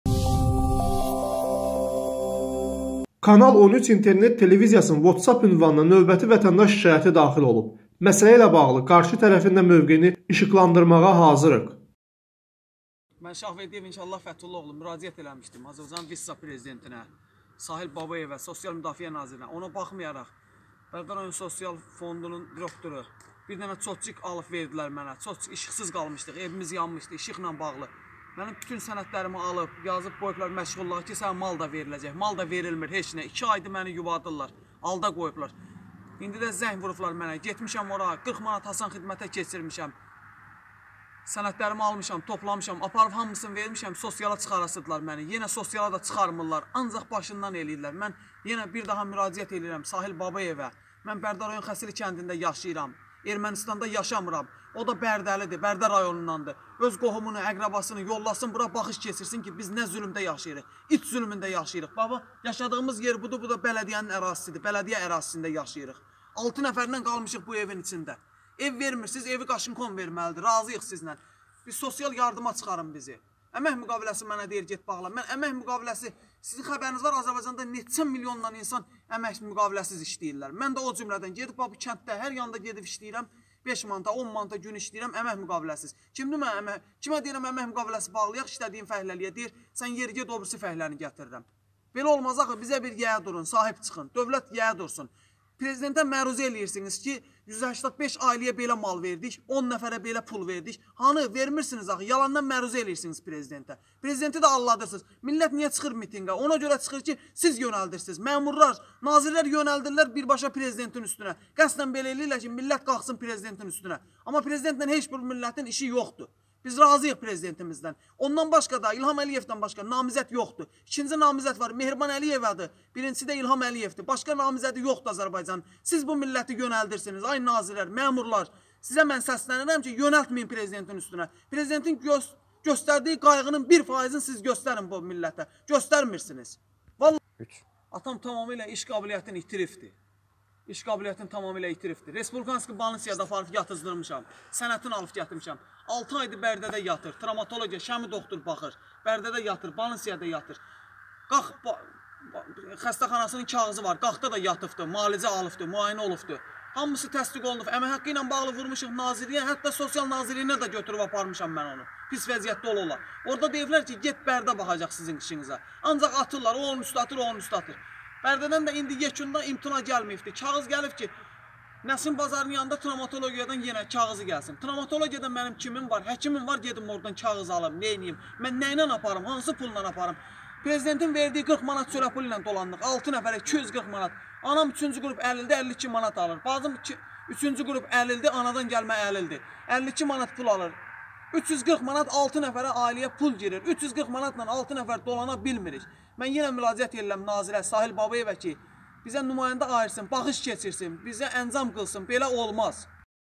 "Məmurlar bilərək xalqa zülm edir ki, insanlar mitinqə çıxıb prezidenti qarşı etiraz etsin"
Kanal 13 internet televiziyasının  vhatsapp ünvanına vətəndaşdan növbəti şikayyət daxil olub. Məsələ ilə əlaqədar olaraq qarşı tərəfin də mövqeyini işıqlandırmağa hazırıq.